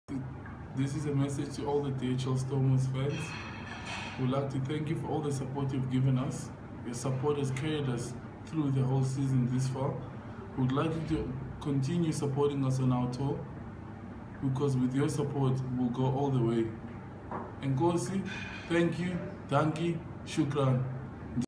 Siya Kolisi's message to the Faithful
The DHL Stormers are grateful to the fans. Without your support they would not have done so well in this competition. They humbly ask you to stand behind their team as they go on their Australasia tour. With your continued support and faith, they can go all the way!